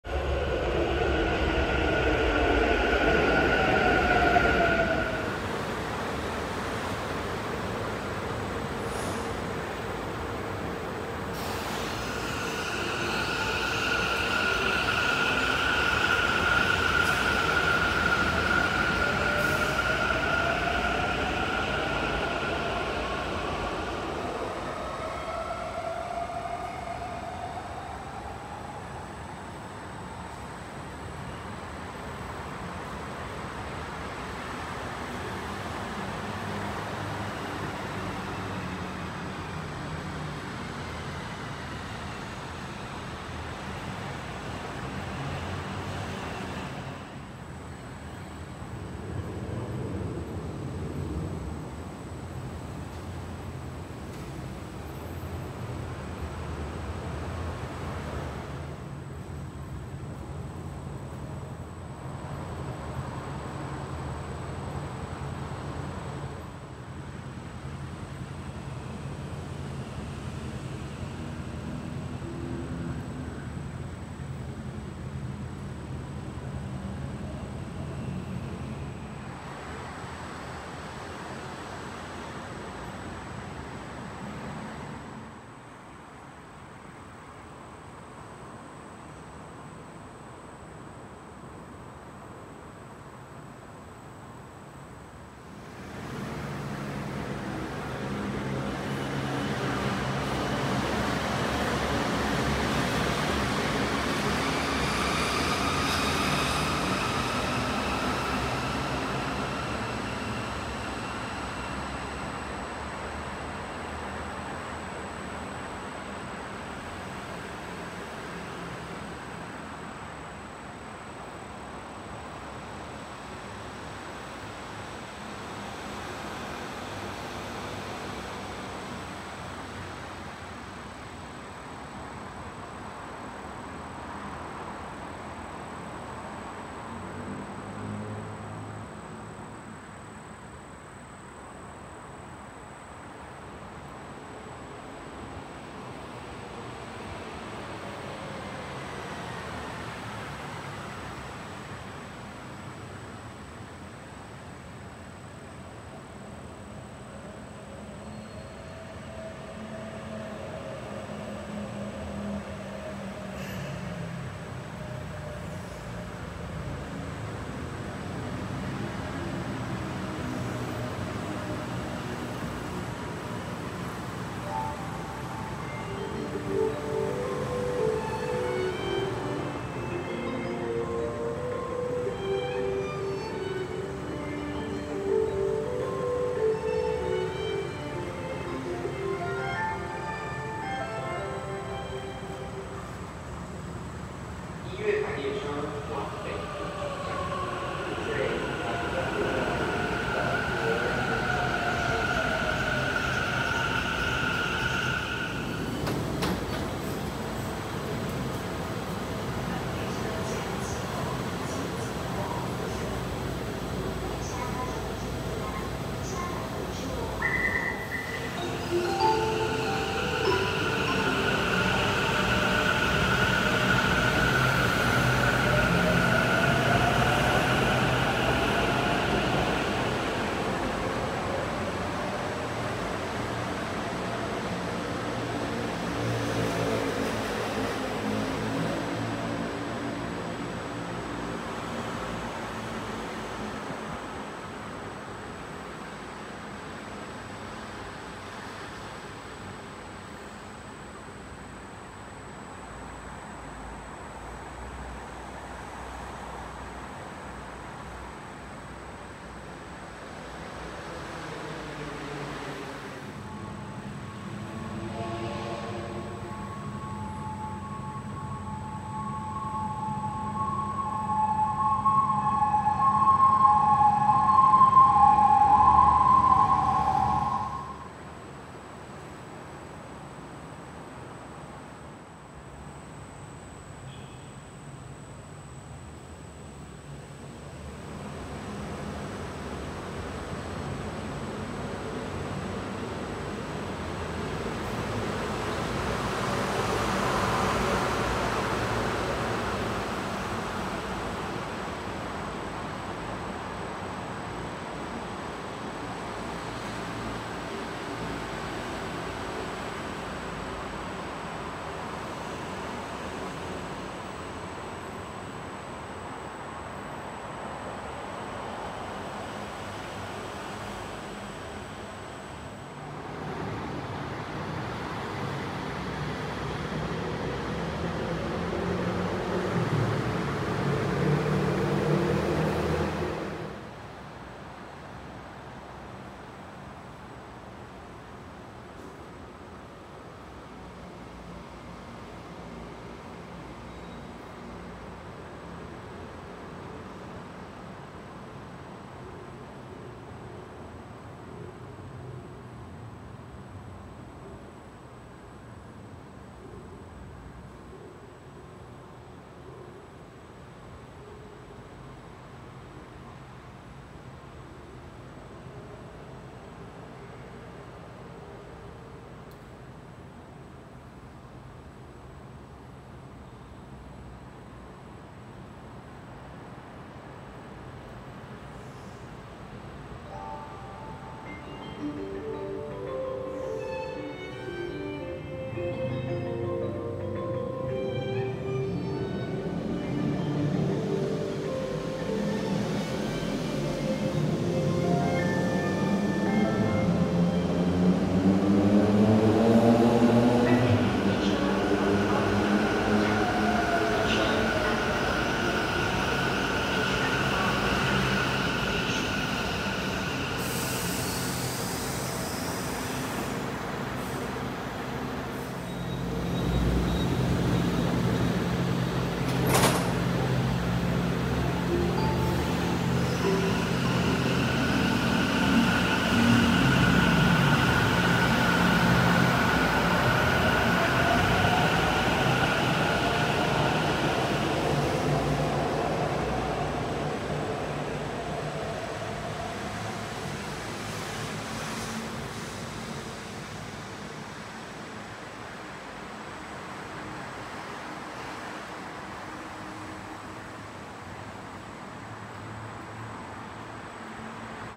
台中捷運(台中MRT) 文心中清站(文心中清駅)・文華高中站(文華高中駅) 月台(ホーム) 列車資訊顯示器(発車標) LED顯示器(LED電光掲示板) 最後一班火車出發後(最終電車発車後)
0:00 文心中清站(文心中清駅)
2:54 接近メロディ
6:20 接近メロディ 最後一班火車(最終電車)
6:56 文華高中站(文華高中駅) 最後一班火車出發後(最終電車発車後)